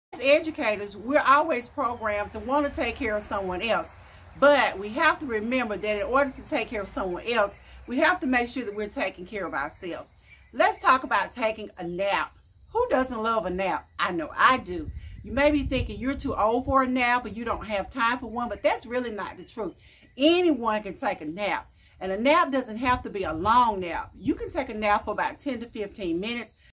Teacher Health Tip: Taking Naps
Our health is important. In order to function effectively at our jobs, we need to make sure we are taking care of ourselves.  Naps are an important part of that process.  This quick video will discuss the benefits.